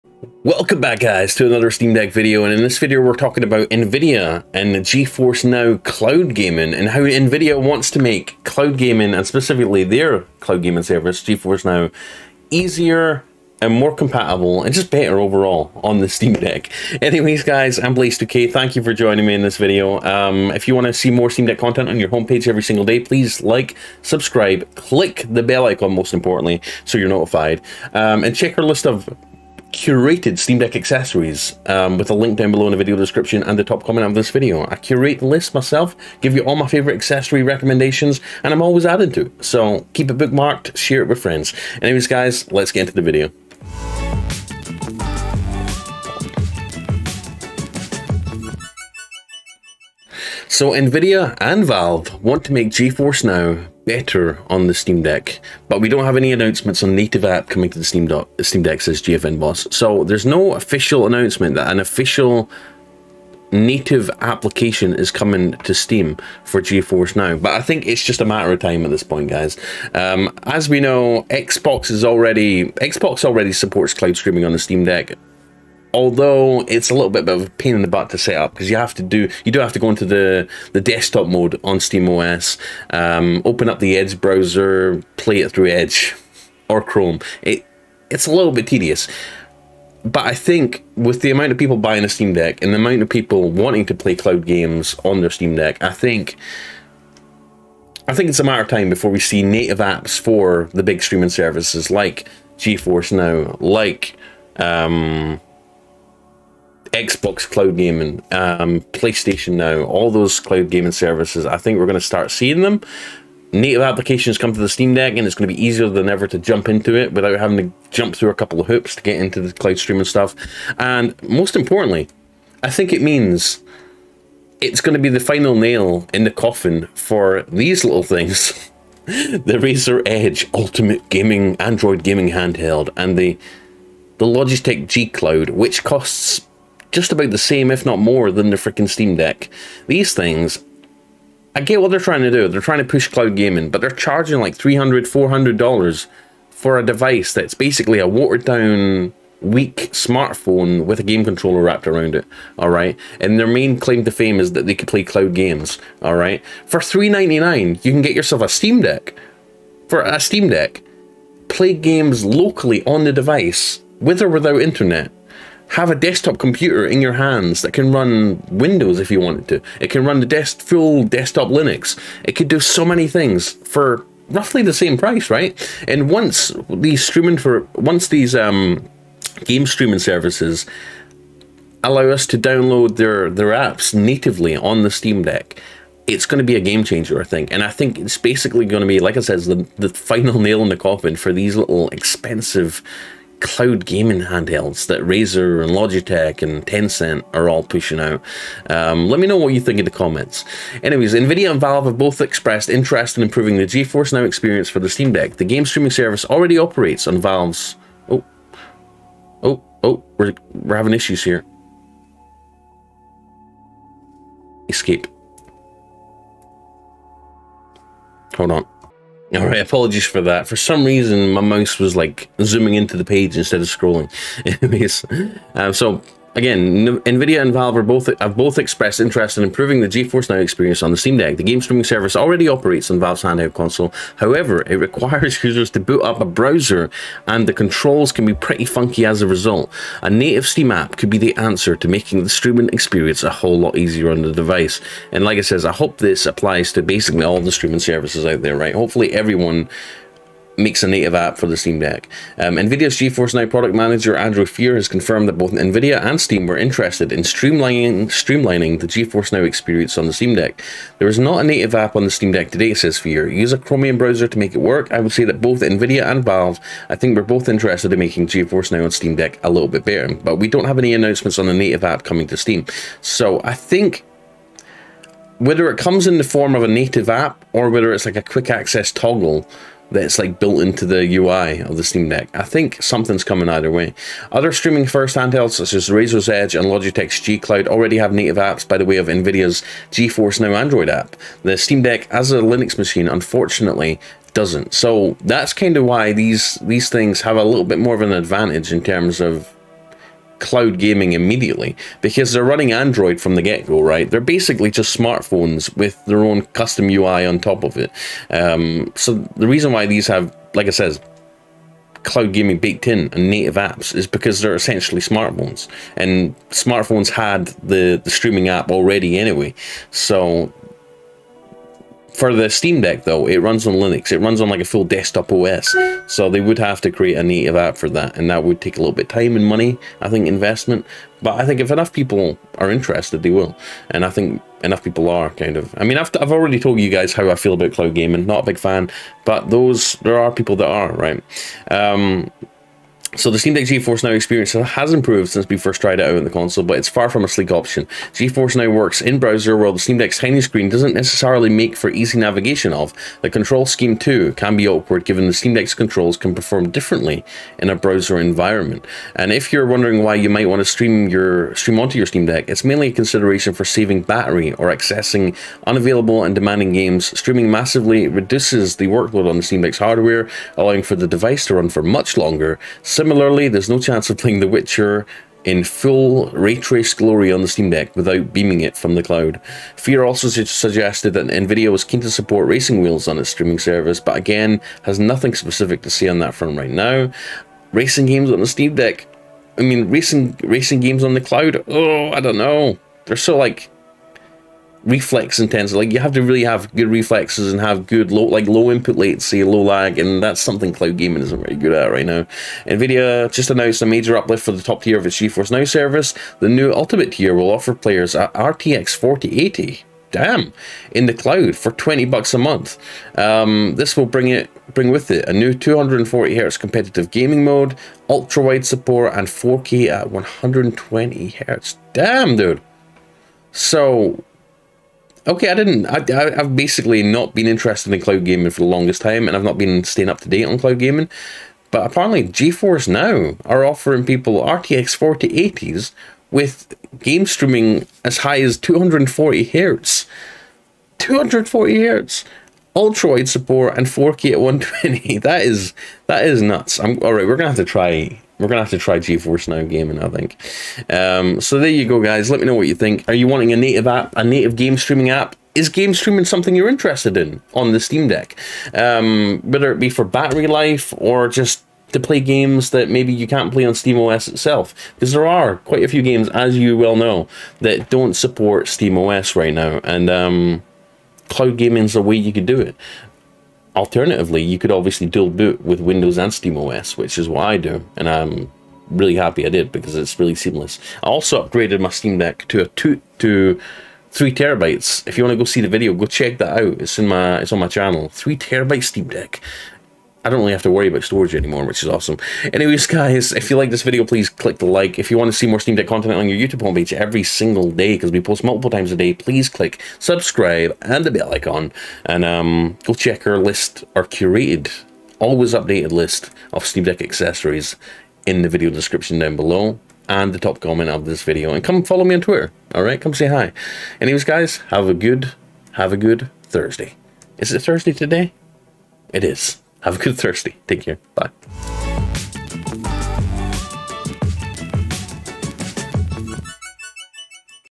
NVIDIA and Valve working on bringing Geforce Now to Steam Deck! Native App coming soon?
Nvidia and Valve have announced that they are working on how to bring Nvidia cloud streaming to the Steam Deck! If Cloud gaming services bring native apps to the Steam Deck does that spell the end of the Razer Edge and Logitech G Cloud Gaming Handheld?

★ My FAVORITE Steam Deck Accessories